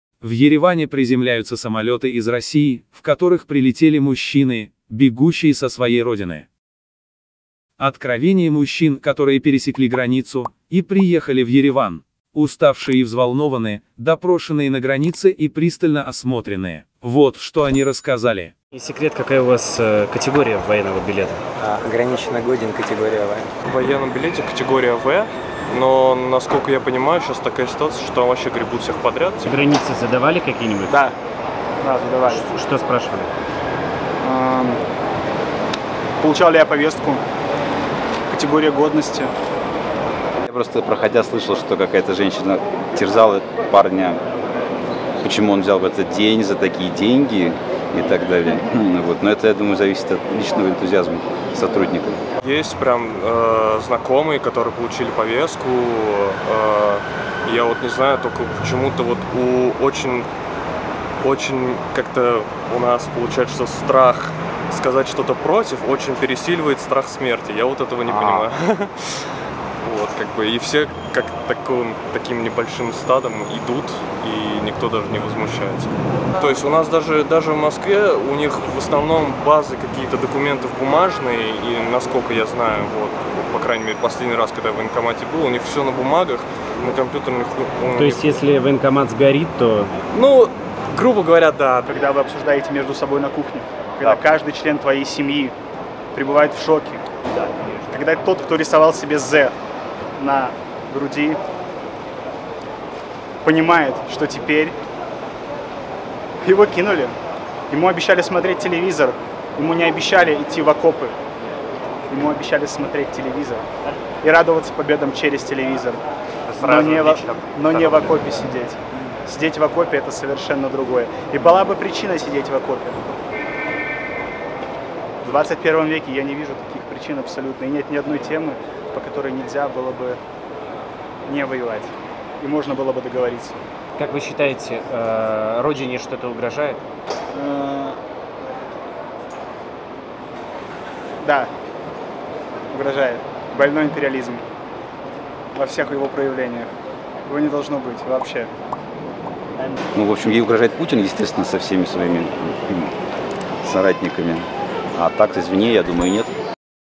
Война в Украине бегут Россияне от мобилизации в Армению
В Ереване приземляются самолёты из России, в которых прилетели мужчины, бегущие со своей родины от мобилизации.
Откровения мужчин, которые пересекли границу, и приехали в Ереван. Уставшие и взволнованные, допрошенные на границе и пристально осмотренные.
Можно их назвать предателями или беженцами?
А этом видео рассказ этих людей.